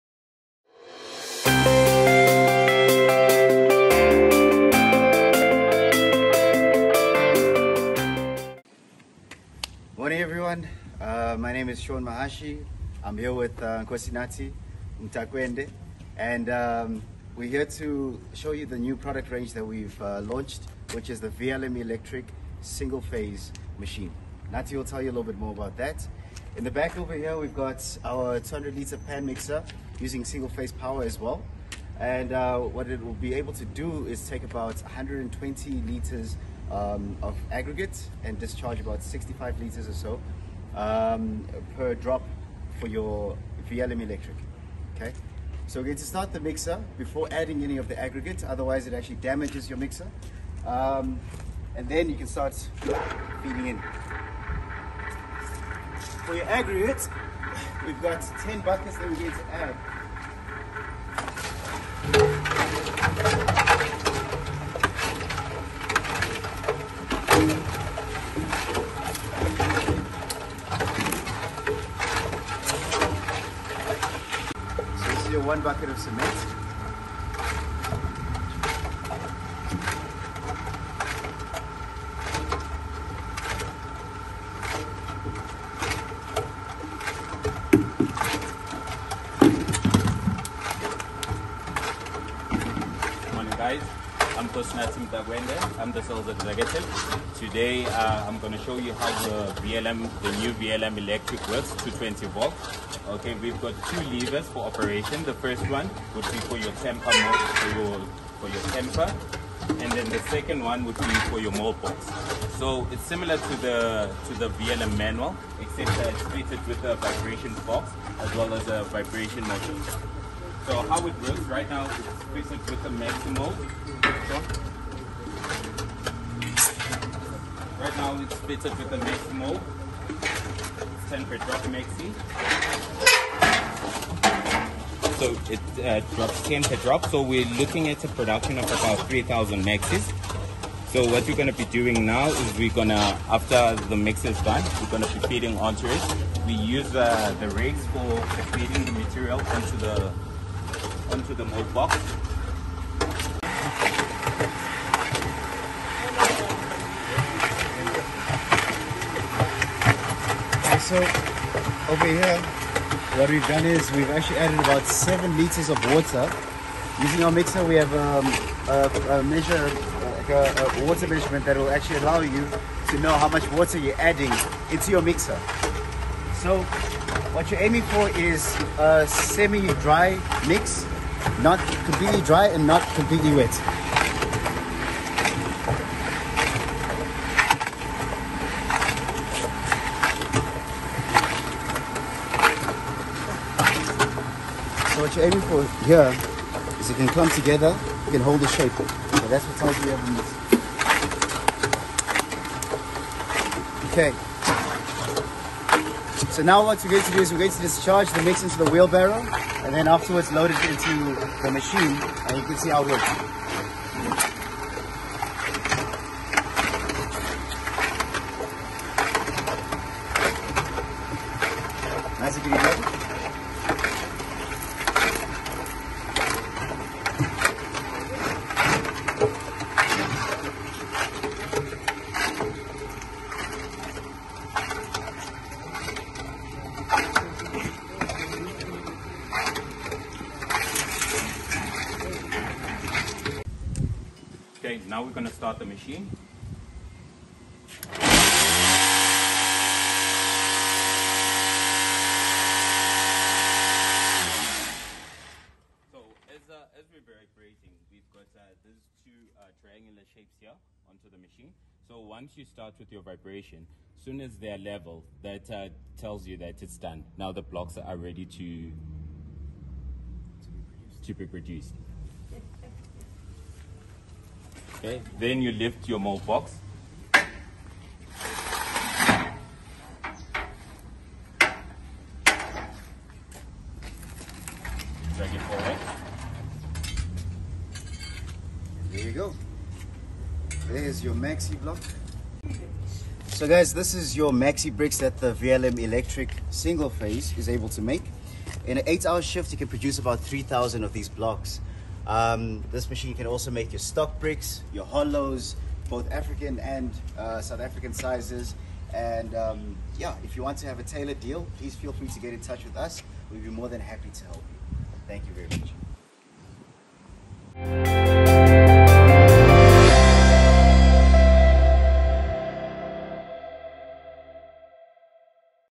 A Demonstration of the new electric VLM Machine
New electric VLM Machine to help expand blockyard production. Here we look at how to operate the VLM machine and how much blocks it can produce.